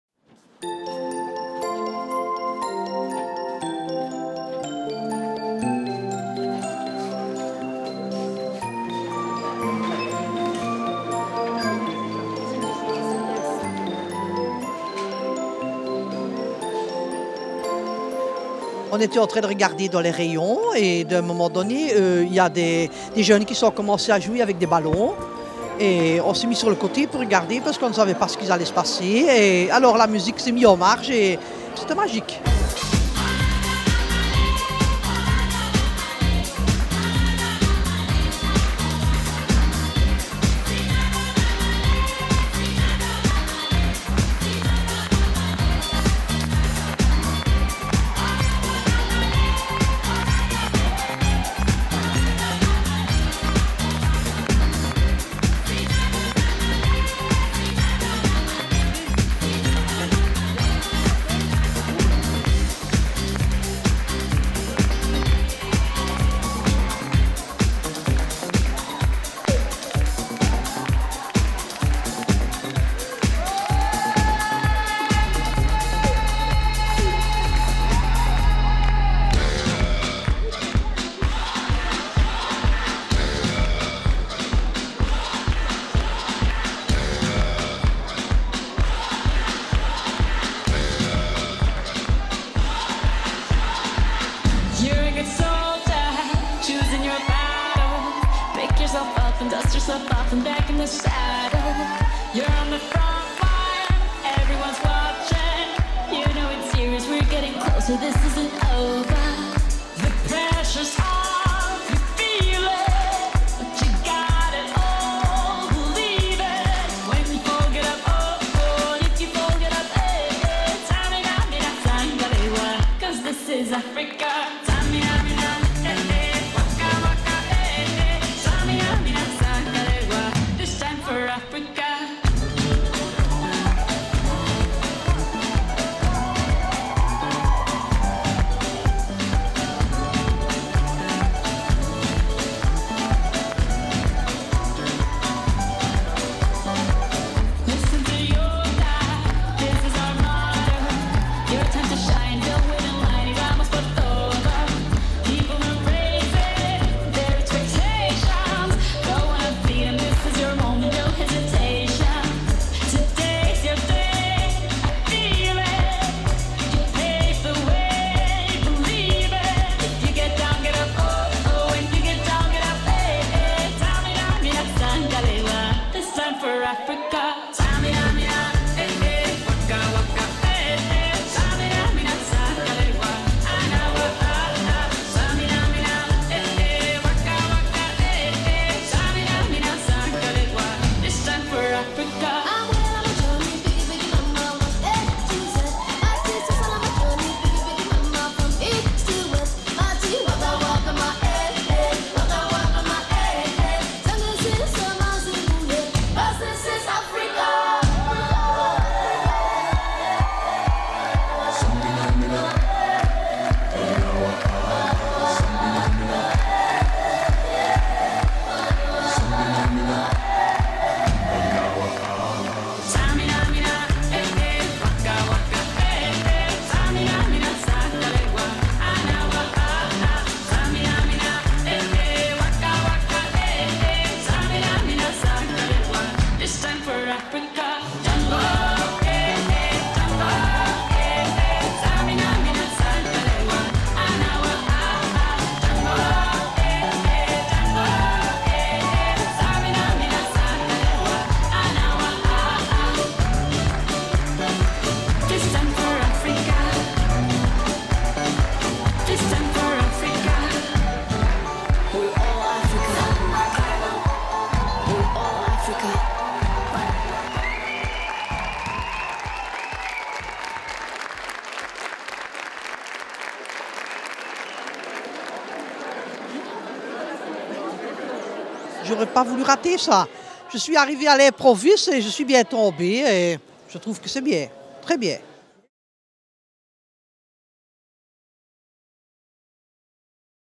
Flashmob Shakira Decathlon Alleur HD
Production - réalisation de la vidéo : Benoît Huc
La version haute définition de la Flash mob des vendeurs de Décathlon Alleur
Décembre 2010